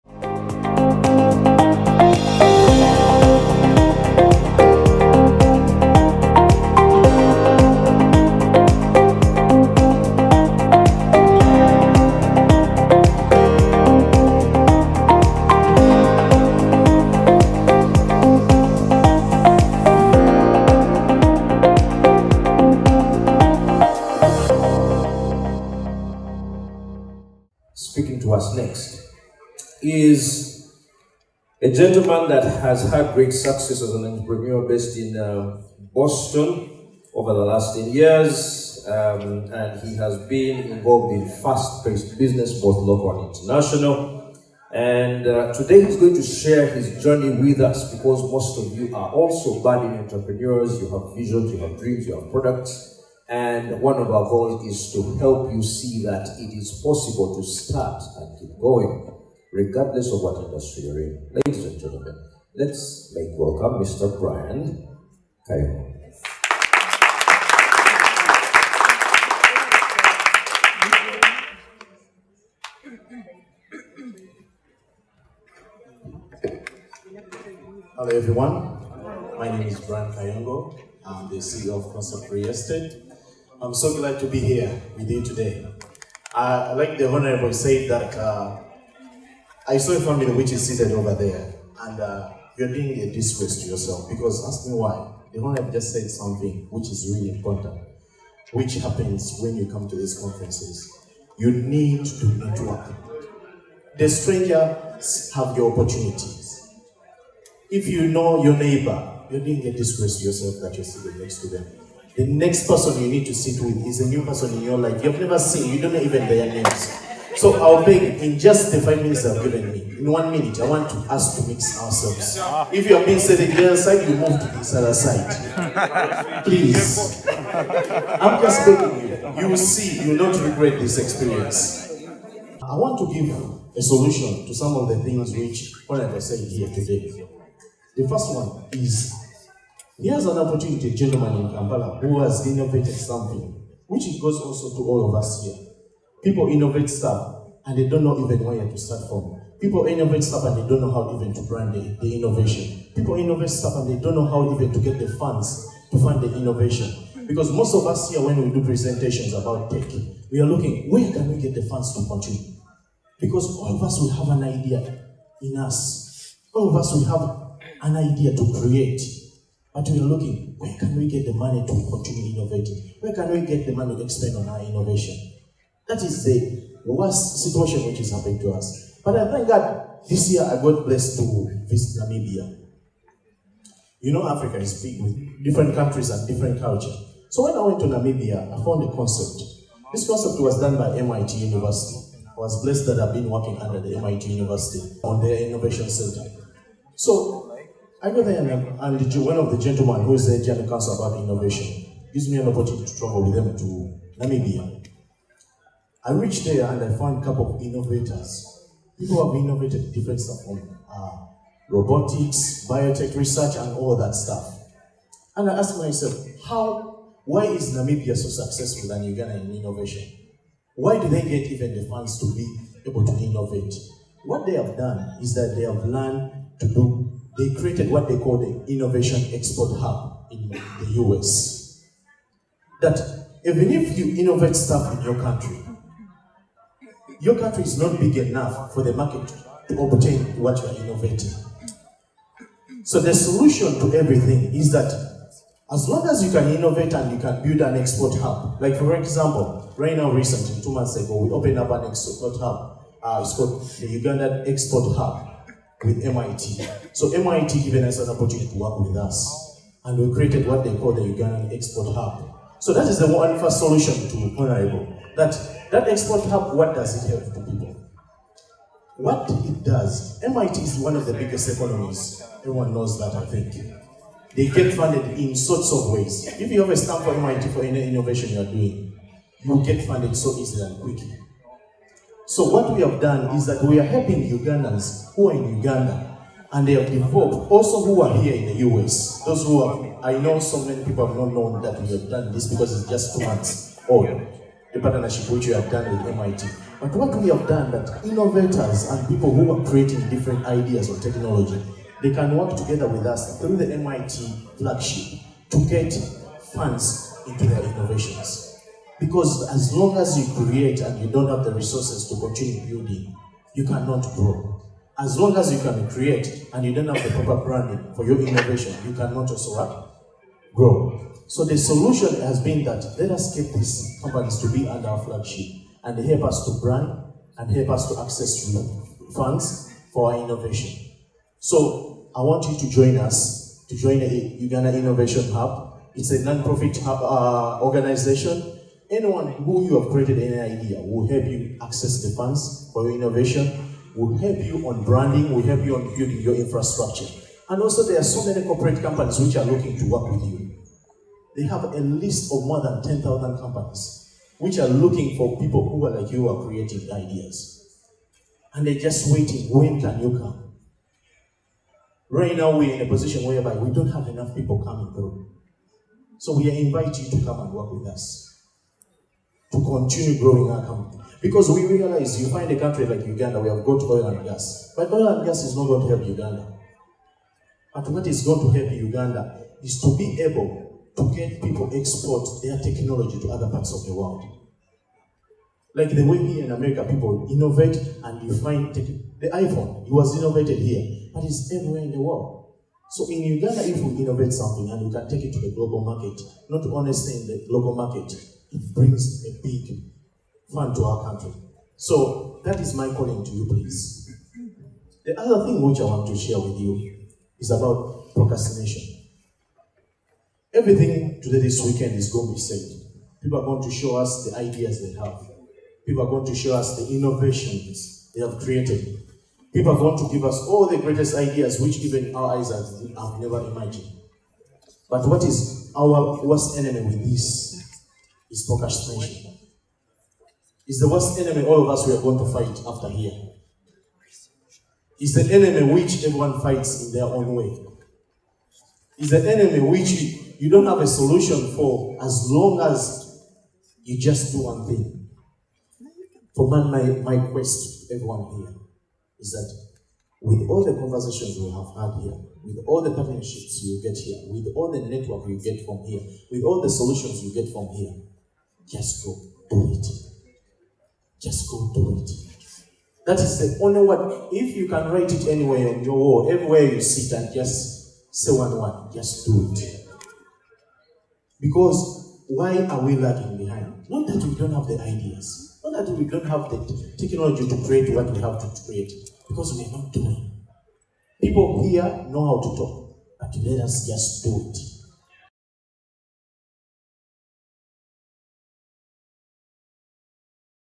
INNOVATION BEYOND BORDERS
At the UNAA Convention 2023, Brian shares his eye-opening experience visiting Namibia, where he discovered the power of export hubs in propelling tech and innovation onto the world stage.